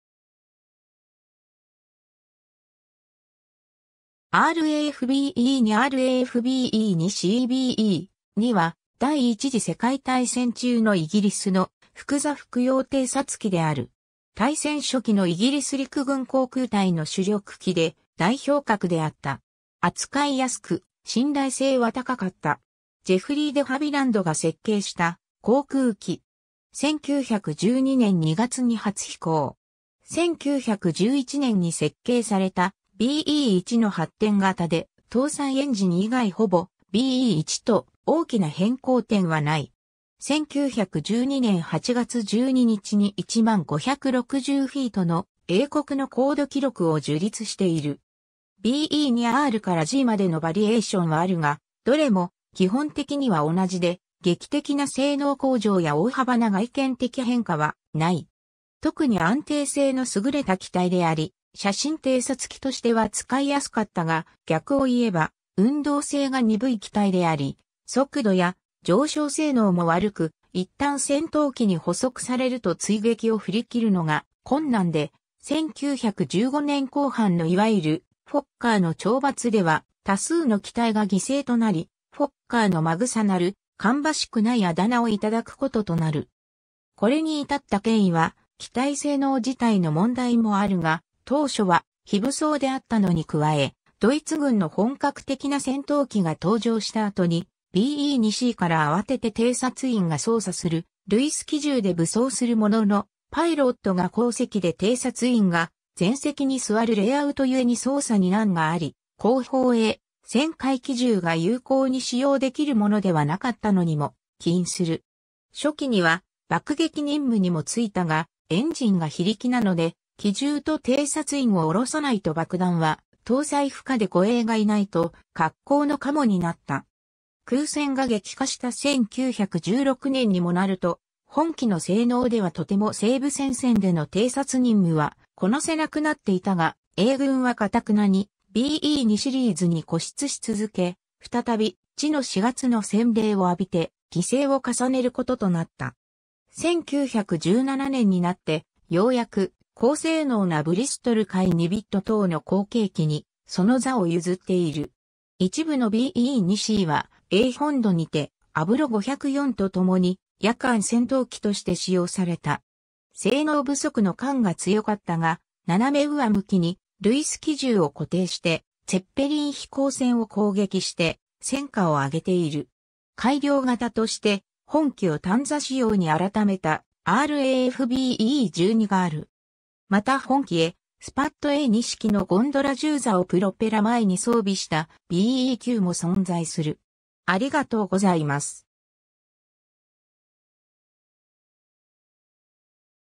RAF B.E.2
RAF B.E.2 RAF B.E.2c B.E.2は、第一次世界大戦中のイギリスの複座複葉偵察機である。
大戦初期のイギリス陸軍航空隊の主力機で代表格であった。
扱い易く、信頼性は高かった。
 ジェフリー・デ・ハビランドが設計した航空機。
1912年2月に初飛行。
1911年に設計されたB.E.1の発展型で、搭載エンジン以外ほぼB.E.1と大きな変更点はない。
1912年8月12日に10,560フィート（3,219メートル）の英国の高度記録を樹立している。
B.E.2aからgまでのバリエーションはあるがどれも基本的には同じで、劇的な性能向上や大幅な外見的変化はない。
 特に安定性の優れた機体であり写真偵察機としては使い易かったが、逆を言えば「運動性が鈍い」機体であり、速度や上昇性能も悪く、一旦戦闘機に捕捉されると追撃を振り切るのが困難で、1915年後半のいわゆる「フォッカーの懲罰」（Fokker Scourge）では多数の機体が犠牲となり、「フォッカーの馬草（まぐさ）」（Fokker Fodder）なる芳しくない渾名を頂くこととなる。
 これに至った経緯は機体性能自体の問題もあるが、当初は非武装であったのに加え、ドイツ軍の本格的な戦闘機が登場した後に、B.E.2cから慌てて偵察員が操作するルイス機銃で武装するものの、パイロットが後席で偵察員が前席に座るレイアウト故に操作に難があり、後方へ旋回機銃が有効に使用出来る物ではなかったのにも起因する（常識的な機銃配置である、パイロットと偵察員の席を前後入れ換えて、旋回機銃を有効にすることは、なぜか最後まで改善されなかった）。
 初期には爆撃任務にも就いたが、エンジンが非力なので機銃と偵察員を下ろさないと爆弾は搭載不可で護衛がいないと恰好のカモになった。
 空戦が激化した1916年にもなると、本機の性能ではとても西部戦線での偵察任務はこなせなくなっていたが、英軍は頑なにB.E.2シリーズに固執しつづけ、再び「血の4月」の洗礼を浴びて犠牲を重ねることとなった。
1917年になってようやく、高性能なブリストルF.2b等の後継機にその座を譲っている。
一部のB.E.2cは英本土にてアブロ 504と共に夜間戦闘機として使用された。
性能不足の感が強かったが、斜め上向きにルイス機銃を固定してツェッペリン飛行船を攻撃して戦果を上げている。
 改良型として本機を単座仕様に改めたRAF B.E.12がある。
また本機へスパッド A.2式のゴンドラ銃座をプロペラ前に装備したB.E.9も存在する。